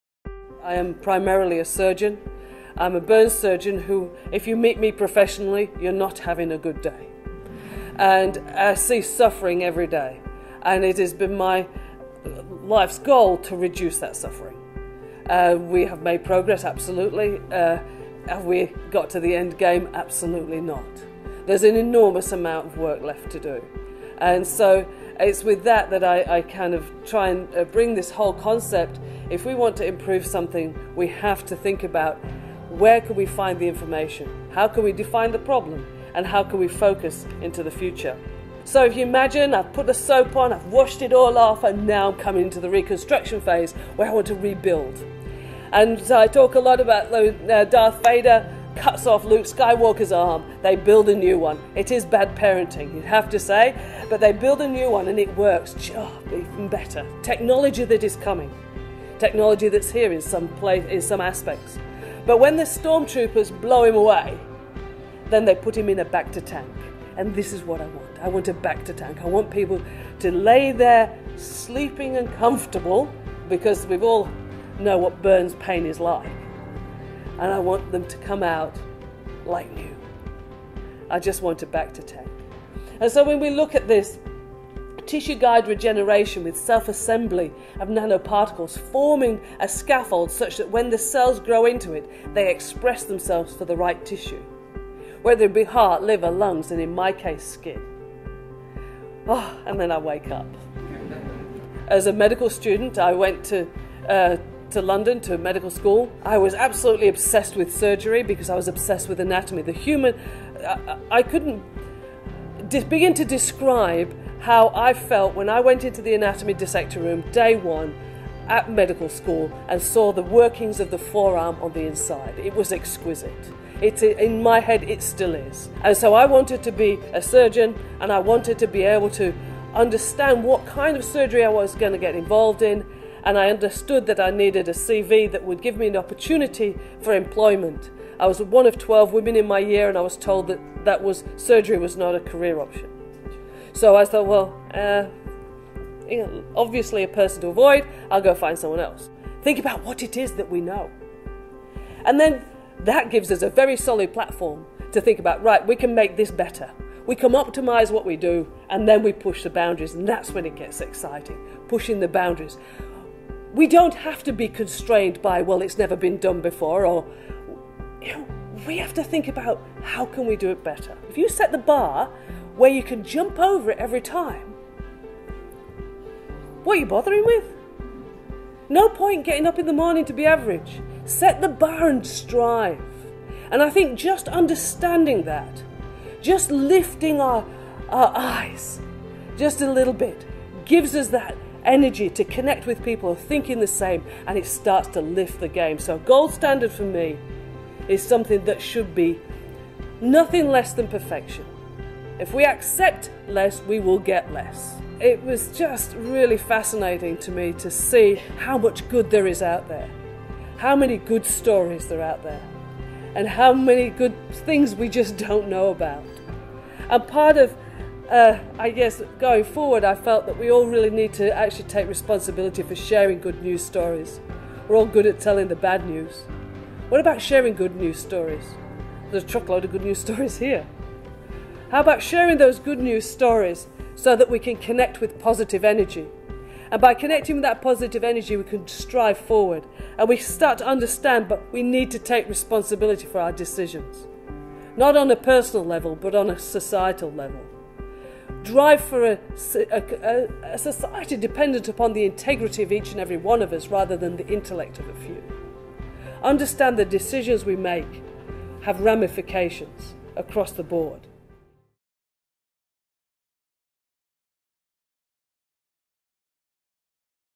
Winthrop Prof. Fiona Wood Lecture Highlights
Watch highlights from Winthrop Professor Fiona Wood's inspirational talk on the challenges we face relating not only to the individual and the health system, but to the whole community.